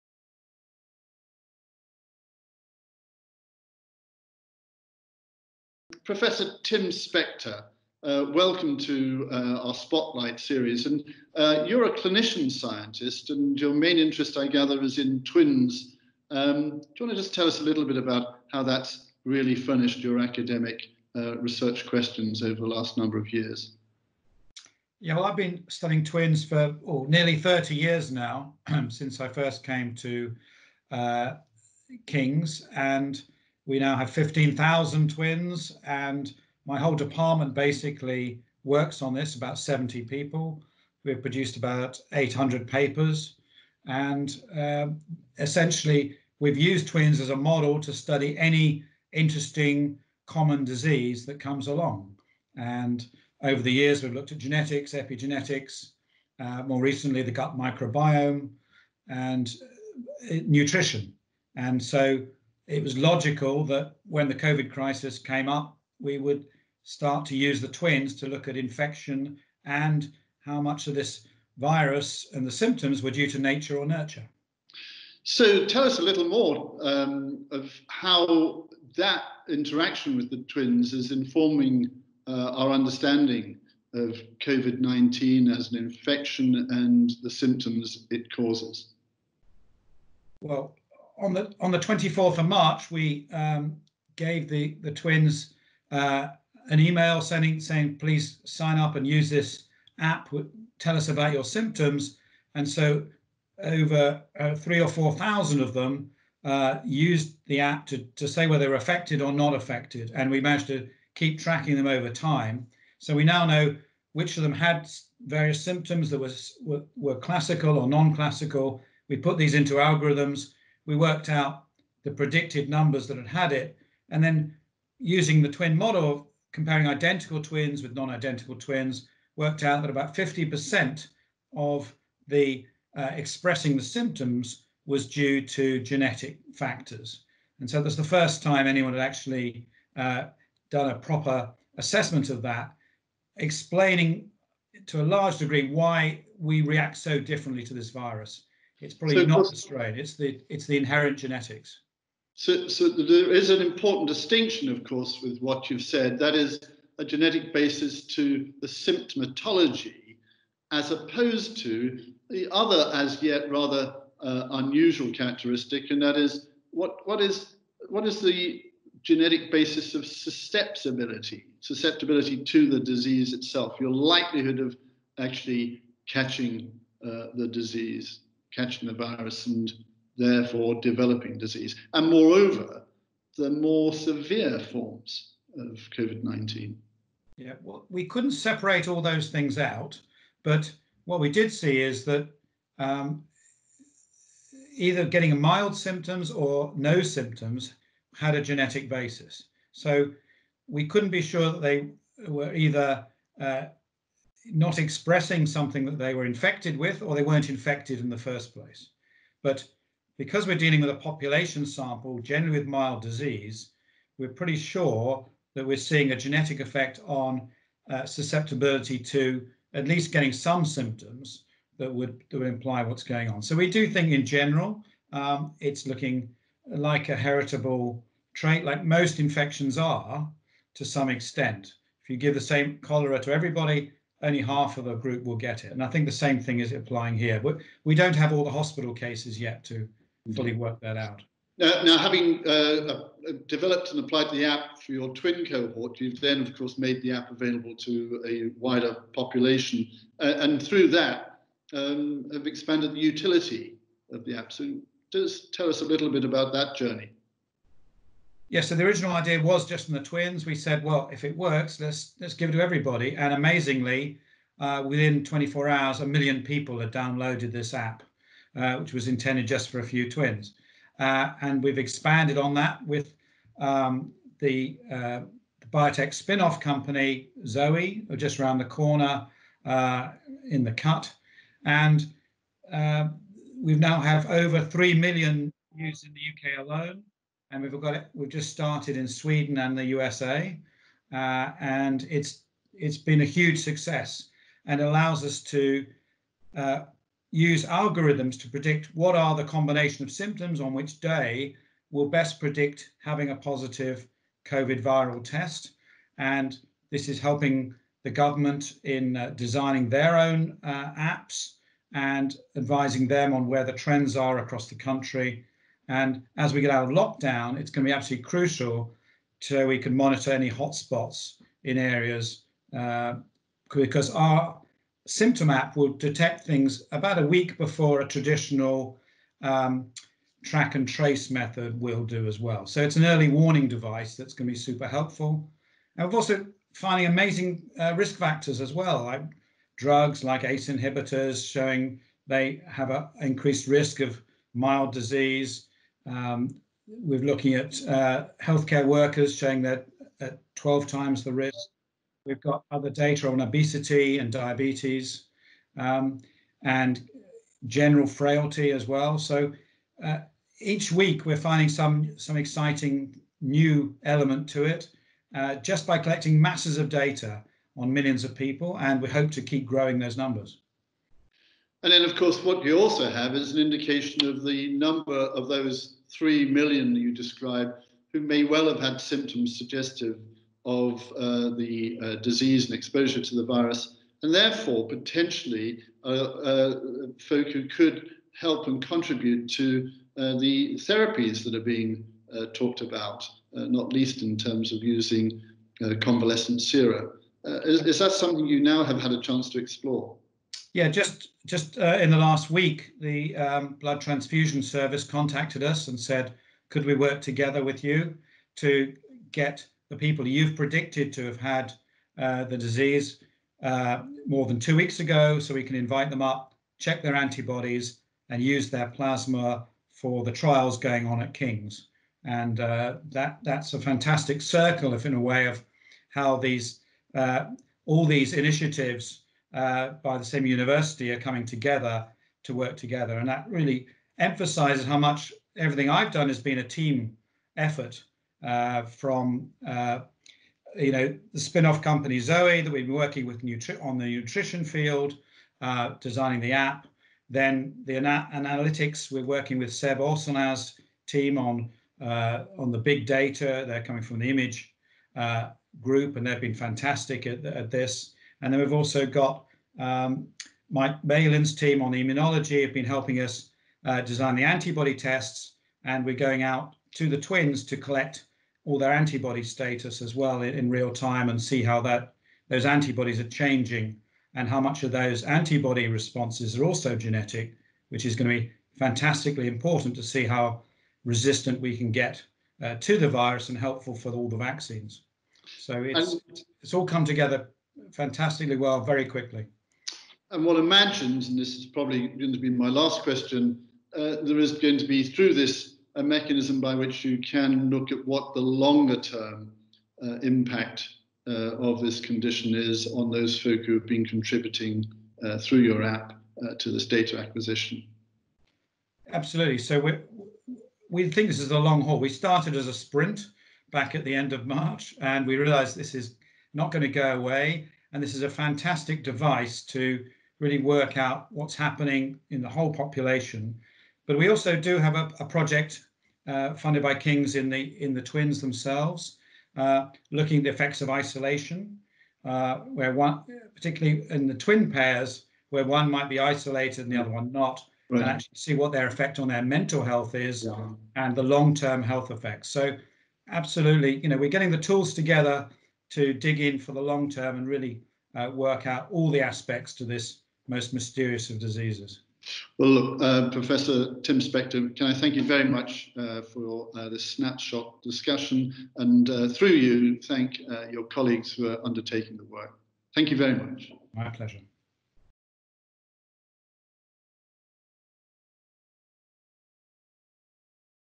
Spotlight on COVID: Tracking the symptoms
Professor Tim Spector, School of Life Course Sciences, discusses the COVID Symptom Tracker app with Professor Richard Trembath, Executive Dean, Faculty of Life Sciences & Medicine.

Data from the app is being used by King's researchers to actively support the global response to coronavirus. It takes just one minute a day to become part of the world’s largest stay-at-home science experiment and help our communities fight this virus.